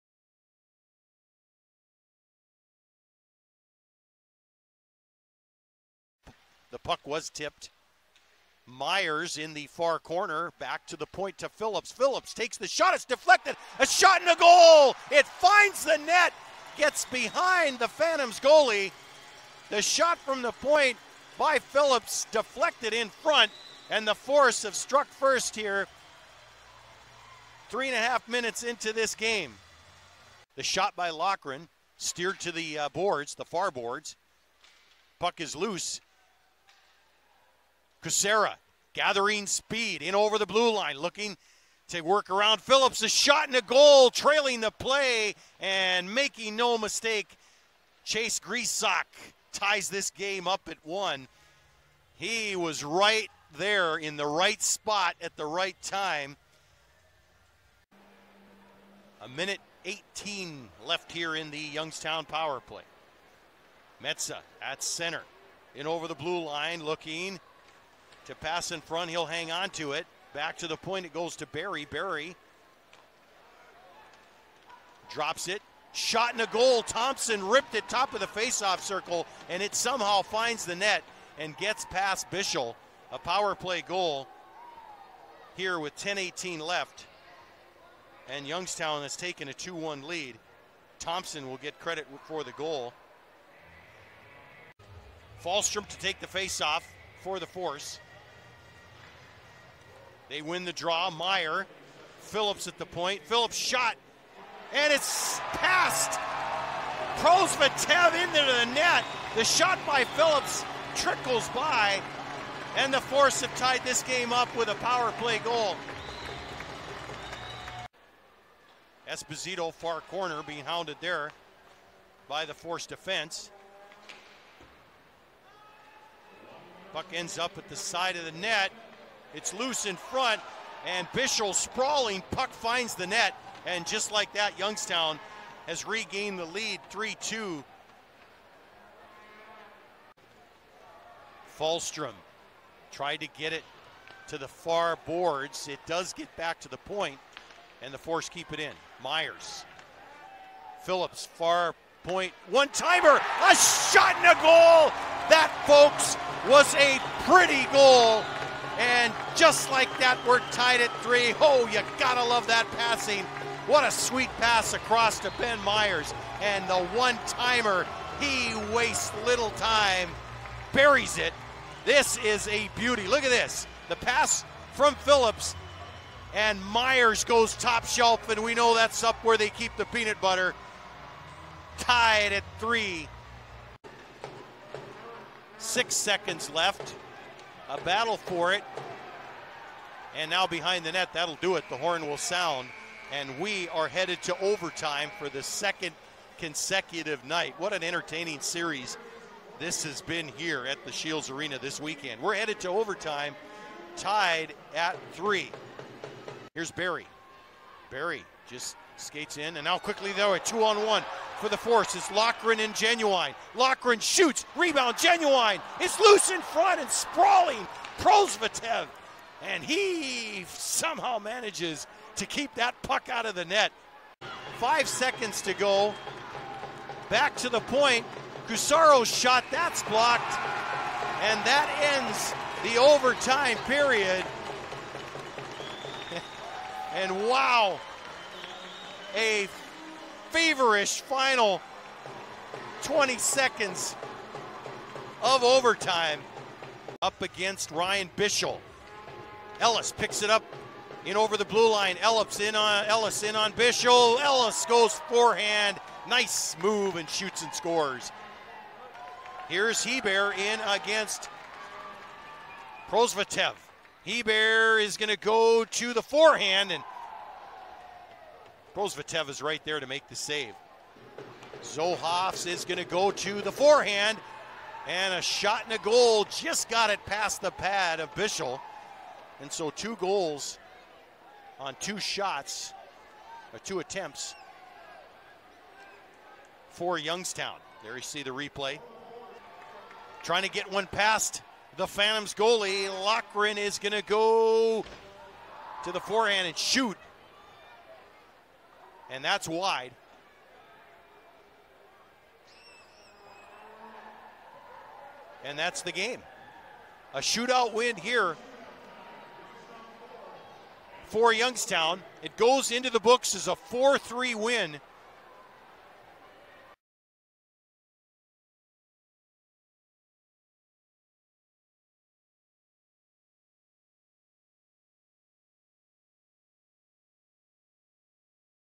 October 14, 2017 Highlights vs Fargo
Highlights from the Phantoms' 4-3 shootout win over the Fargo Force on October 14, 2017